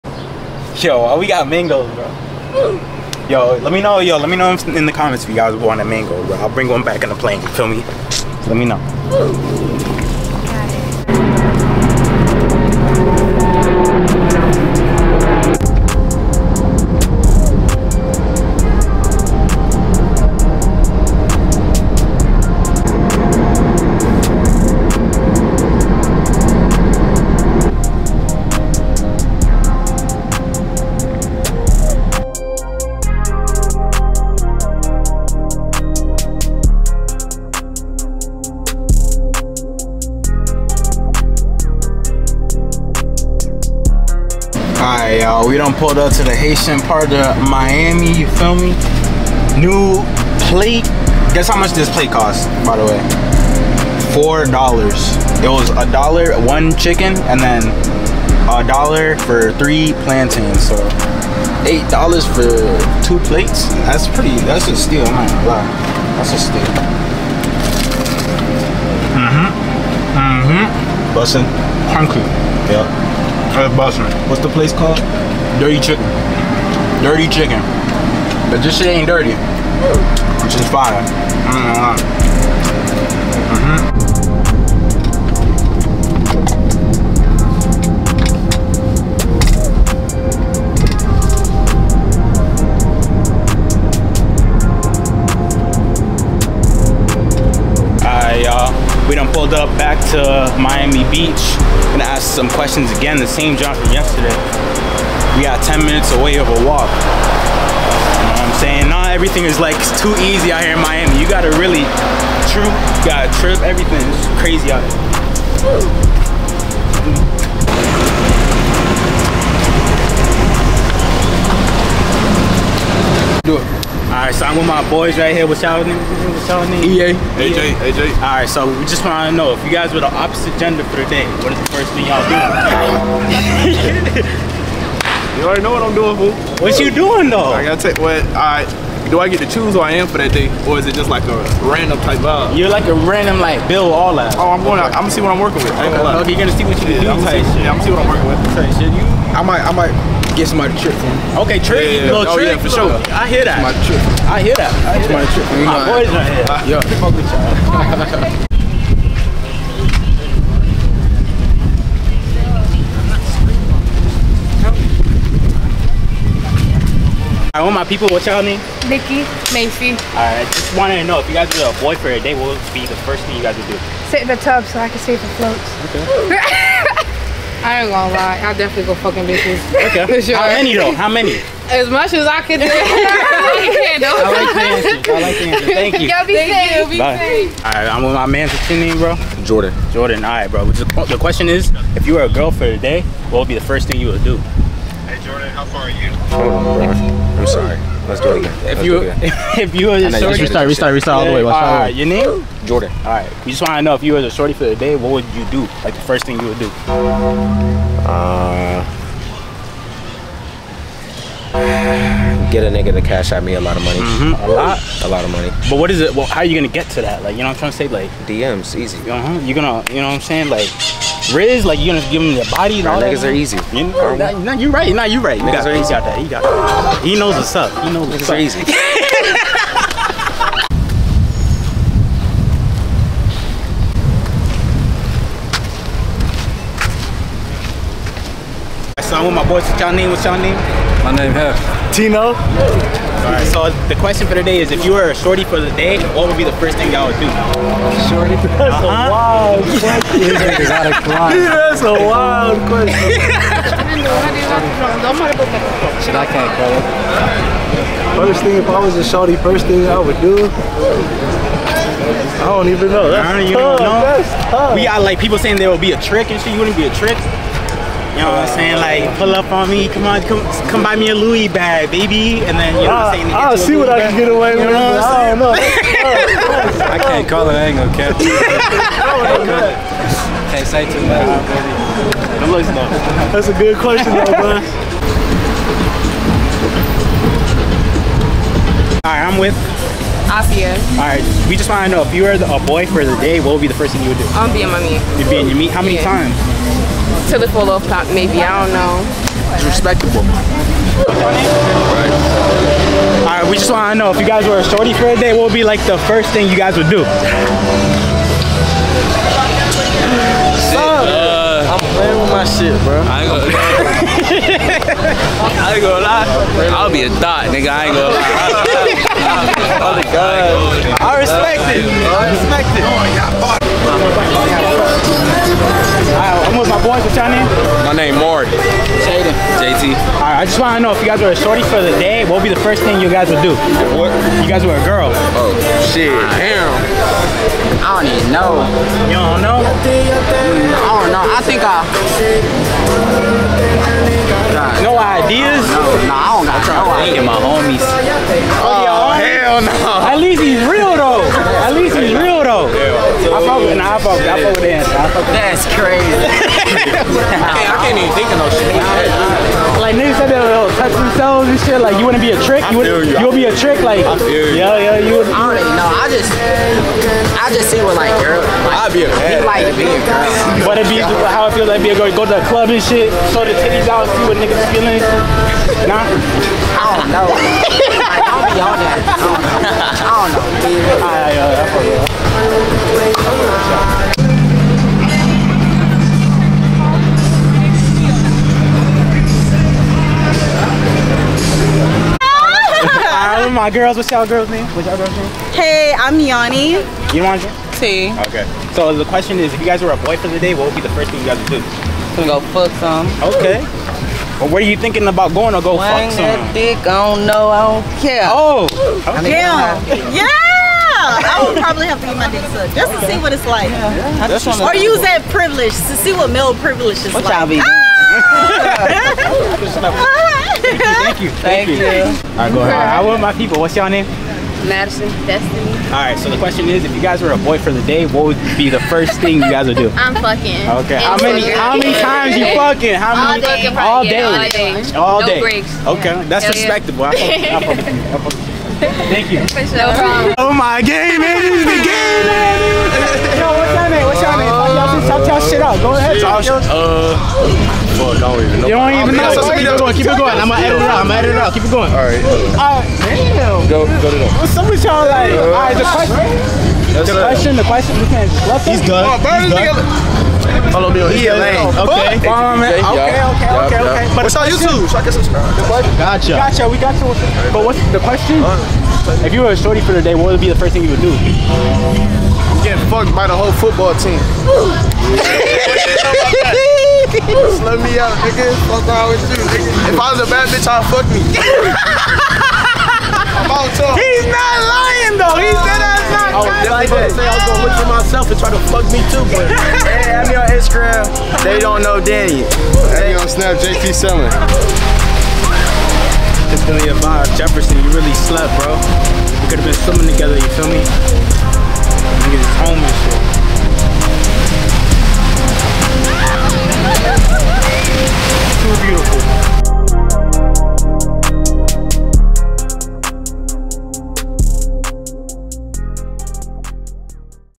KTB SYNDROME : KTB TRIP TO MIAMI DAY 3 ! (PUBLIC INTERVIEWS)
Whats up KTB family in this video we got together for you guys is going to be  us going to Miami Beach and preforming public interviews with strangers we are asking them today what they would do if they were the opposite gender for a day and the reactions and replies we got from these people is crazy so sit back and relax and enjoy this video we got together for our KTB family on our KTB trip to Miami Day 3!!!